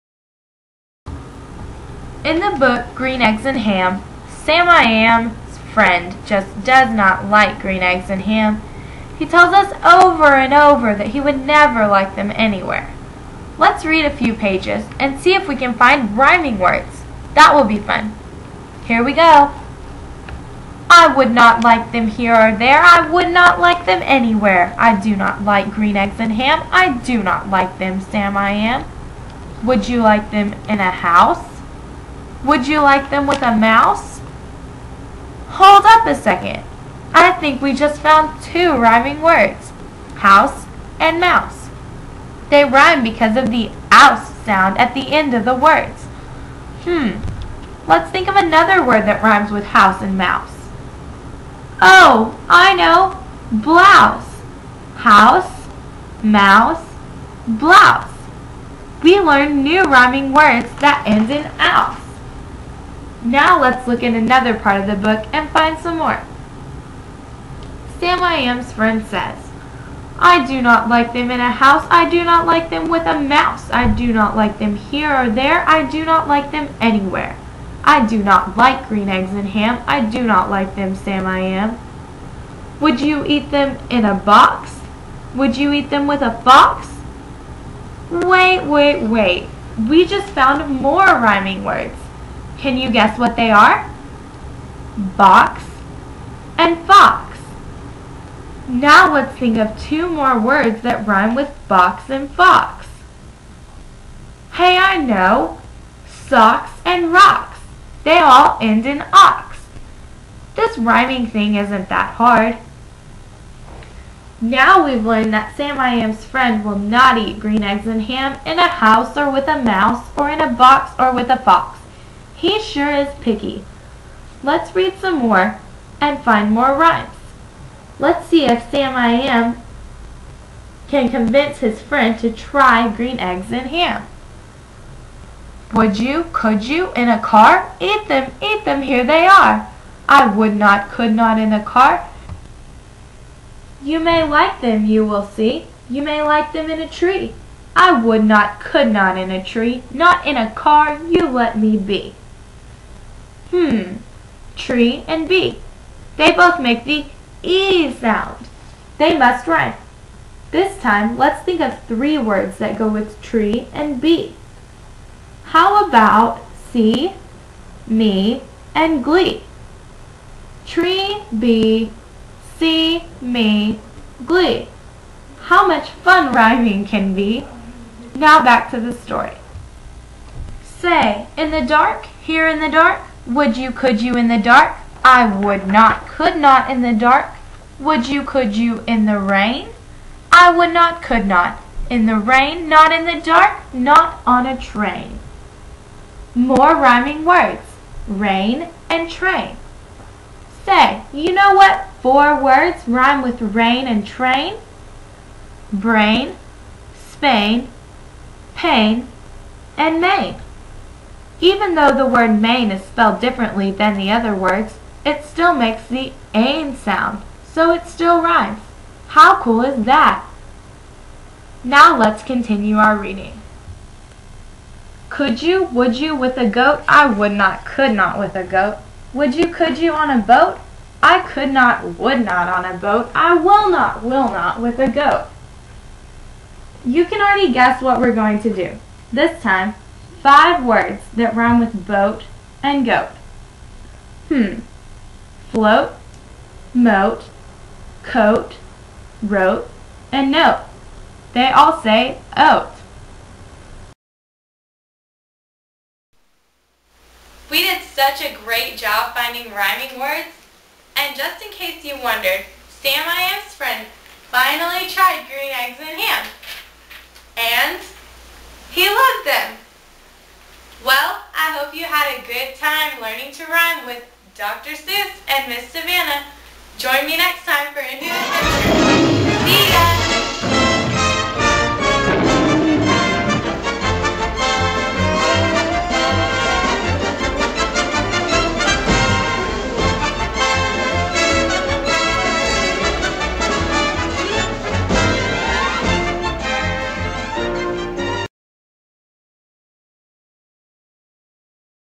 Savannah's Final Video!!!
This is a video about rhyming that could be used in Kindergarten and 1st grade classrooms. I used a Dr. Suess book to demonstrate! Hope you enjoy!!!